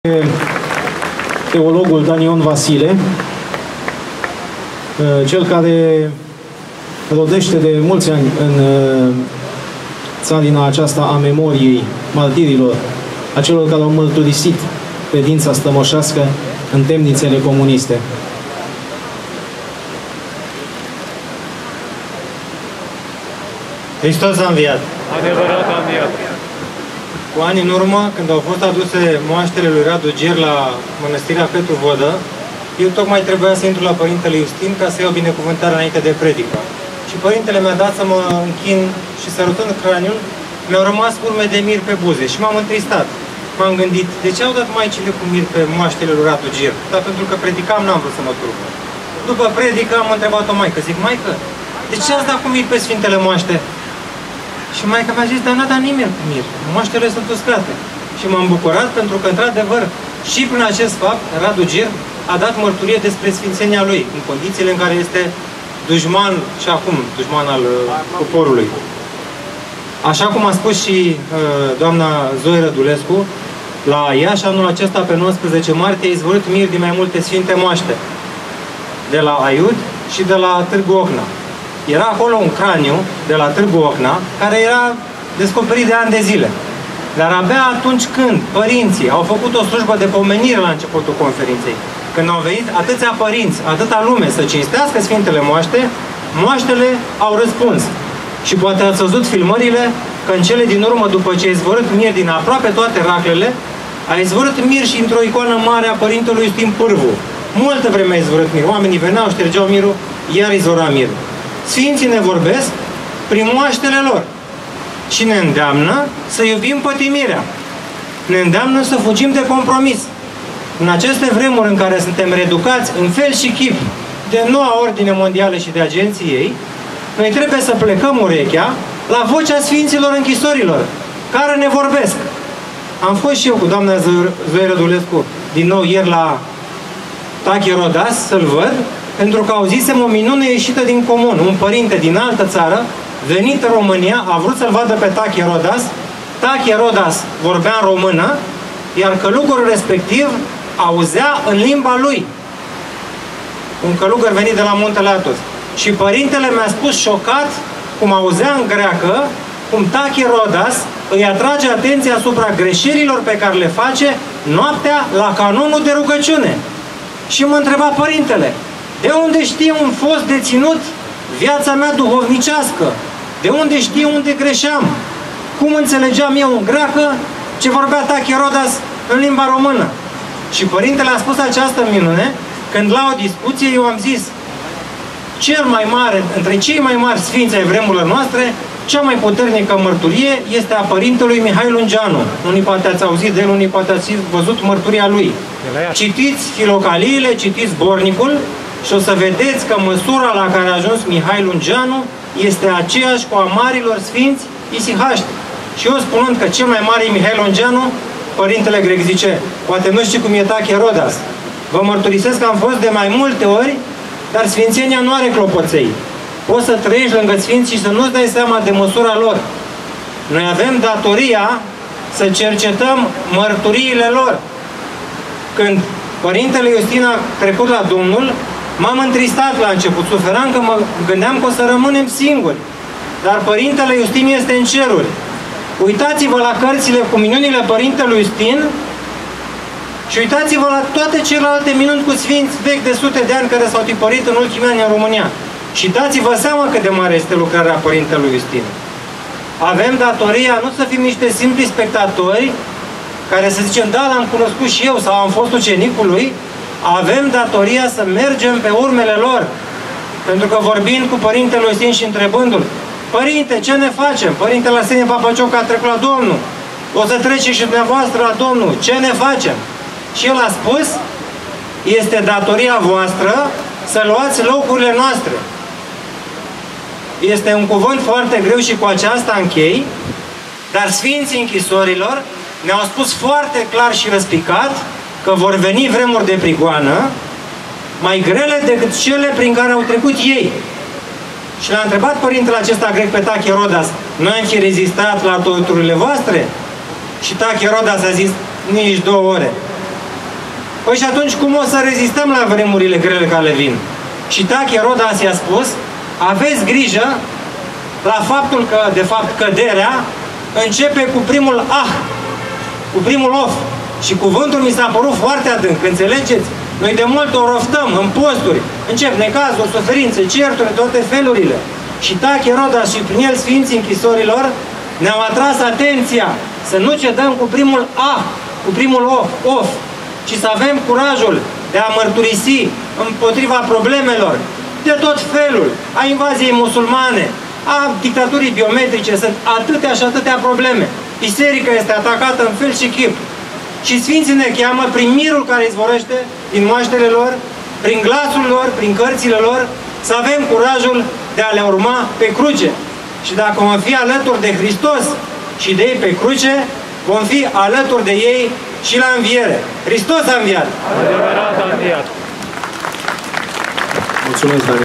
(15) Danion Vasile - Ziua națională de cinstire a martirilor din temnițele comuniste (2019.05.14)
În data de 14 mai 2019, de la ora 19,00, a avut loc în Piața Revoluției (Palatului Regal) din Capitală a doua ediție a manifestării memoriale dedicate martirilor temnițelor comuniste, conform Legii 127/2017. Această lege a instituit ziua de 14 mai ca „zi națională de cinstire a martirilor din temnițele comuniste”.

În expunerea de motive a Legii 127/2017, se arată că această zi a fost aleasă în amintirea celui mai mare val de arestări politice din istoria României (noaptea de 14 mai 1948). Circa zece mii de tineri, foarte mulți dintre ei studenți, au fost arestați în acea noapte fatidică, în baza unui plan al autorităților comuniste, umplând pușcăriile și lagărele de exterminare.

Mai jos, imagini și înregistrarea video a evenimentului.